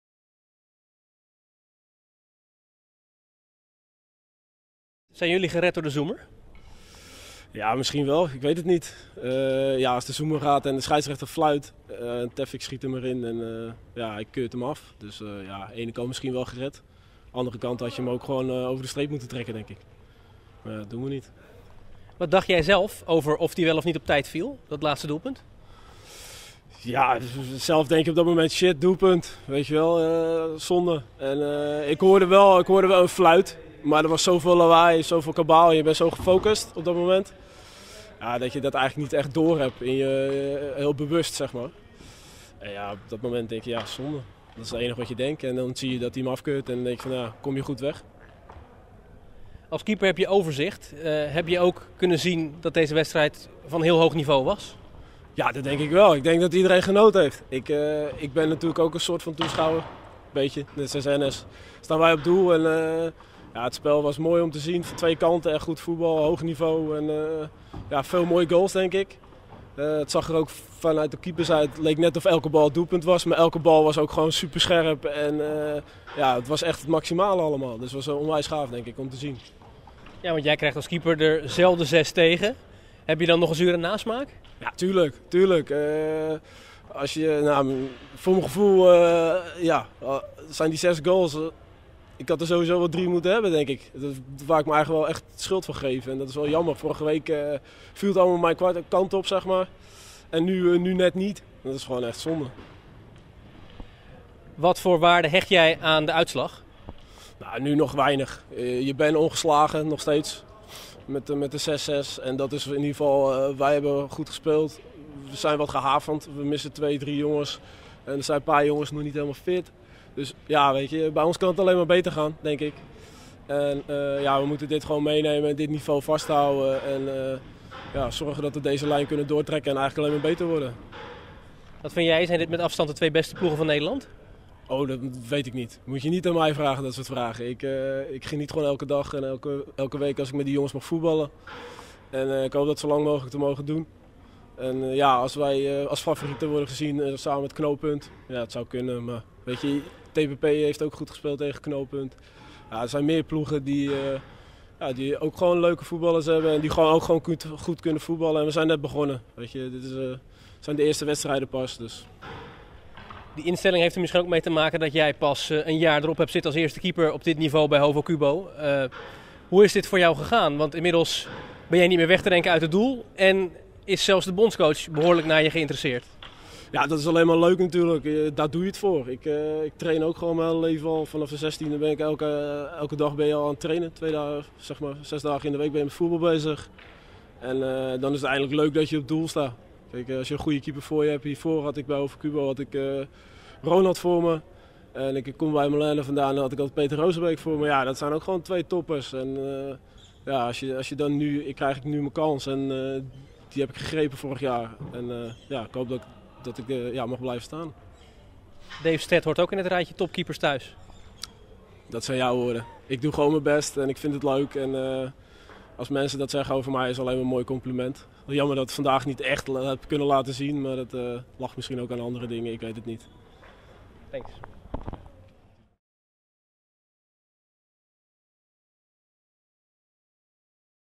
Reactie Dave Stet na Hovocubo 't Knooppunt
Bekijk hier een interview met Dave Stet na afloop van Hovocubo tegen 't Knooppunt.